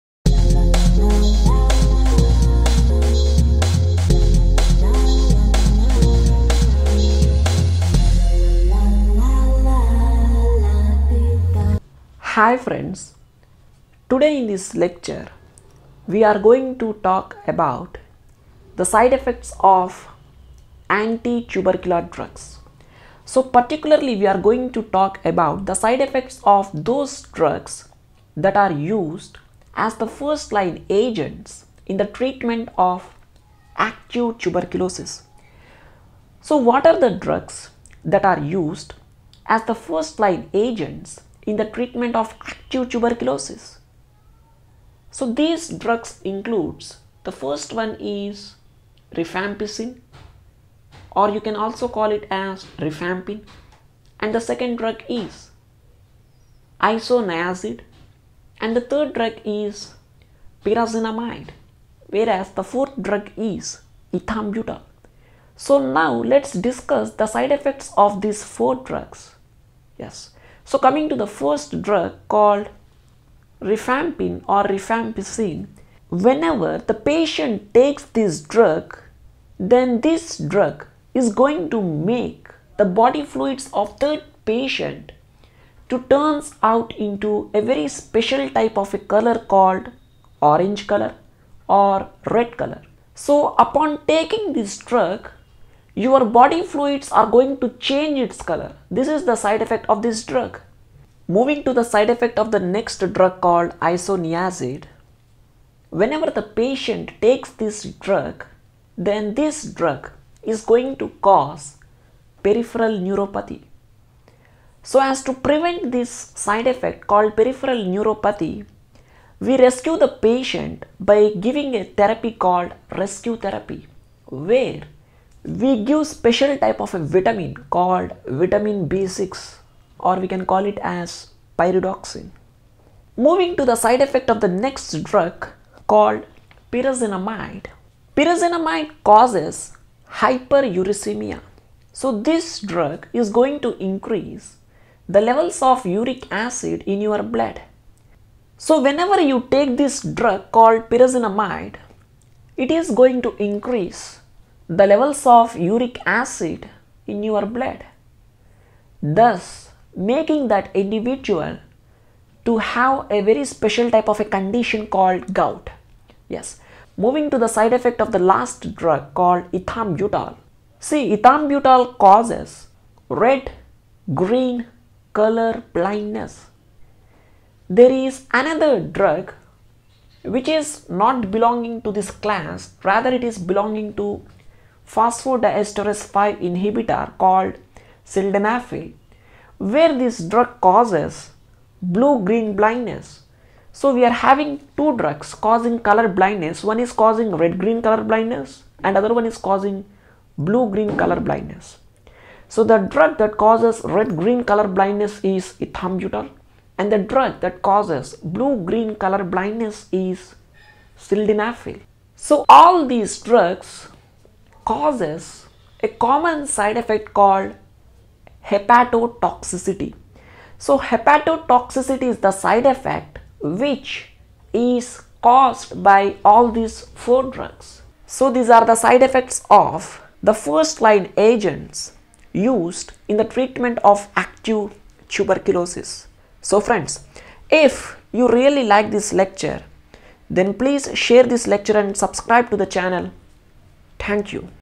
Side effects of Anti Tubercular drugs | RIPE
In this video, we briefly talk about the side effects of Anti-tubercular drugs, particularly on Rifampicin, Isoniazid, Pyrazinamide, and Ethambutol.

About: Concept Clear is an Educational Medical YouTube Channel, where you will find Medical lectures. New Video is Posted Every Wednesday and Sunday.